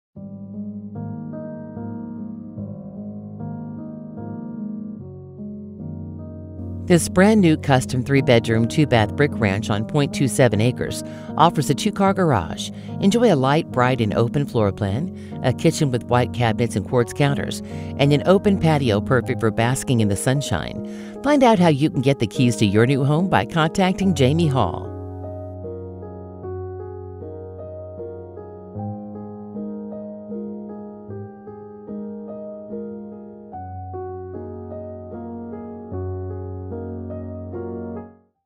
263 N Woodridge Drive Pittsboro, IN | MLS# 21870378
263 N Woodridge Drive, Pittsboro, IN

CUSTOM BRAND NEW 3 BDRM, 2 BAÂ BRICK RANCH on .27 acres w/2 CAR GAR in QUAINT LOW MAINT NGHD in PEACEFUL PITTSBORO!Â This home was being built for a buyer that added LOTS of UPGRADES but that Buyer was unable to perform so next buyer gets those upgrades instead. LIGHT BRIGHT and AIRY OPEN FLOORPLAN! K